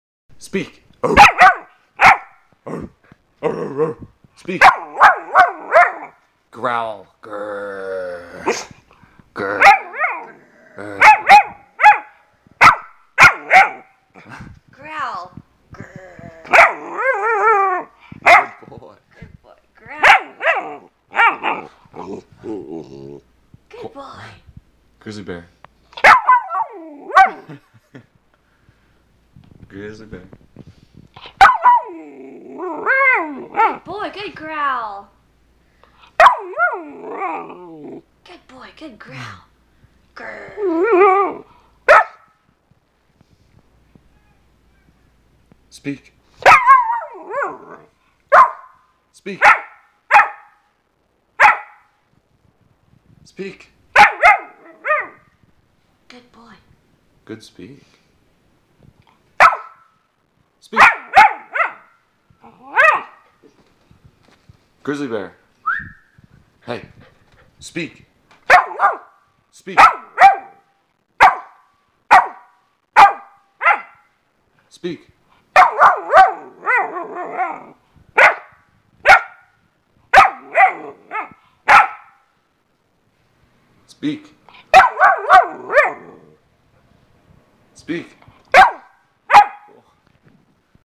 Chocolate lab puppy speaking
'Grizzly Bear' the Chocolate Labrador retriever learning to speak